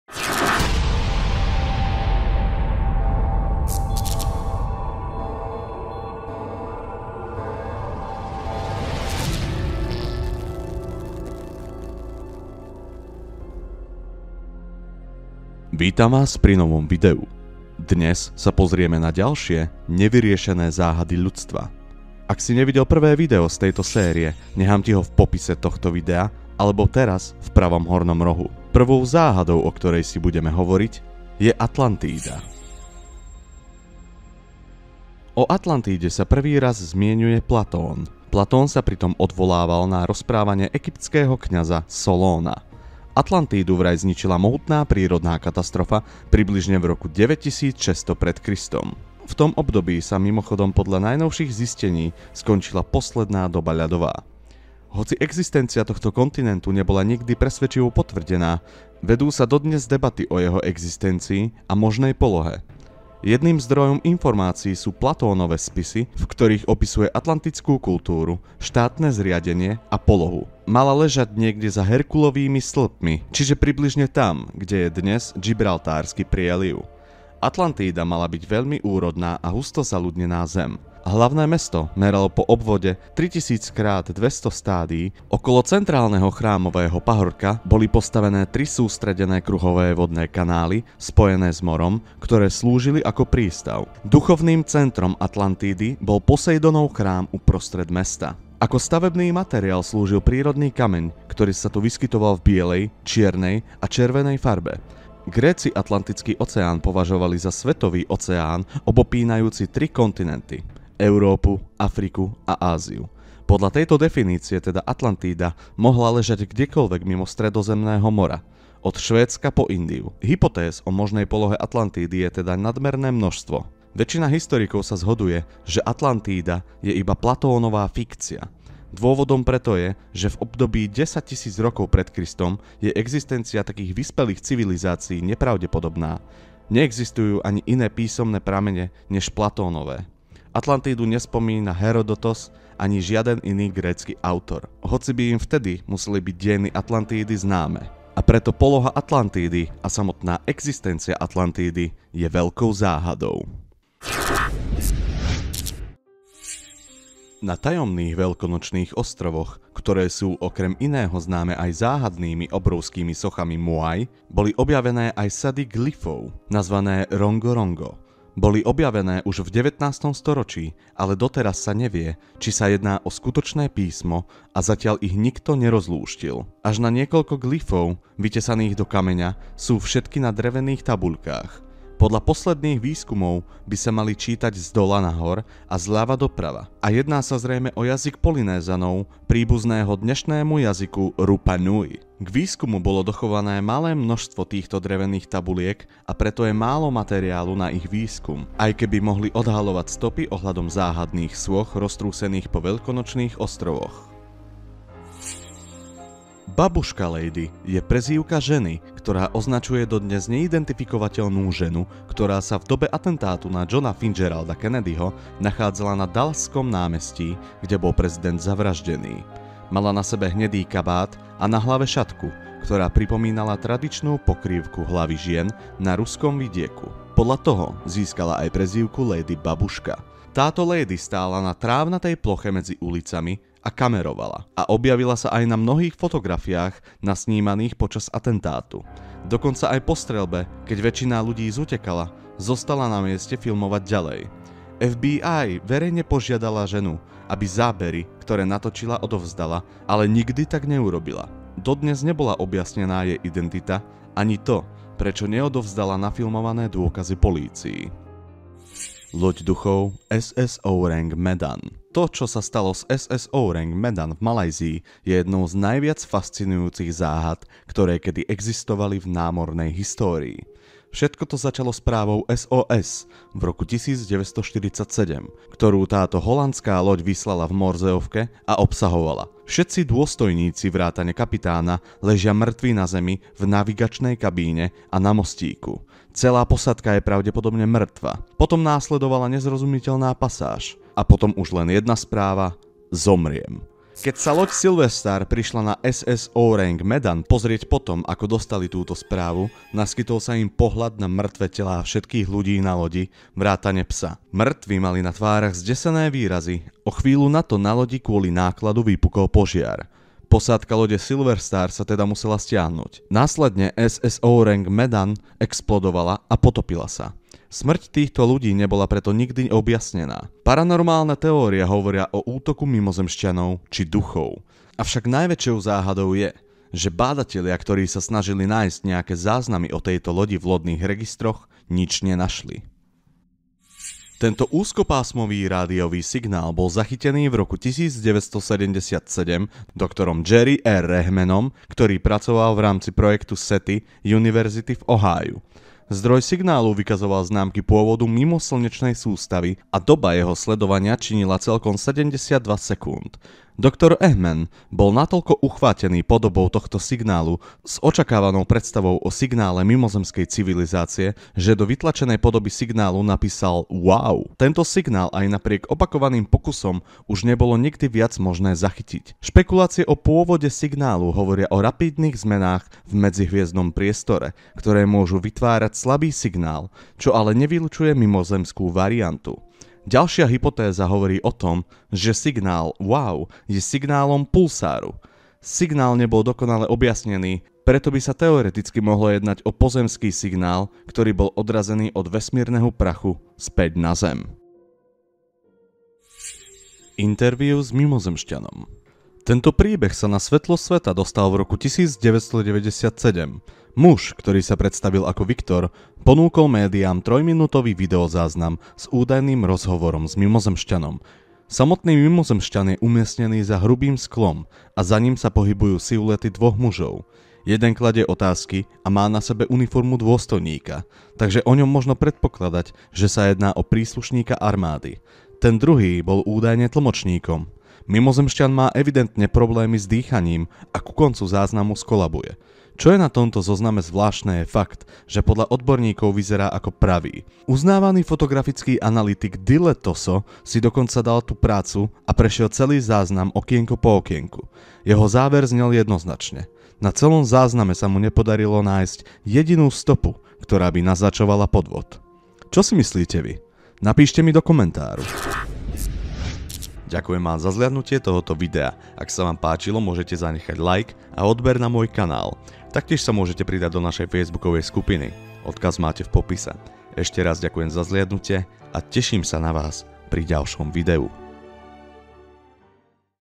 Nevyriešené ZÁHADY ľudstva - [Druhá časť]
Náš svet je plný ZÁHAD, ktoré neboli nikdy vyriešené. Dnes sa pozrieme na 5 nevyriešených a tajomných záhad ľudstva ;)

✅Ak sa ti video páči nezabudni ODOBERAŤ✅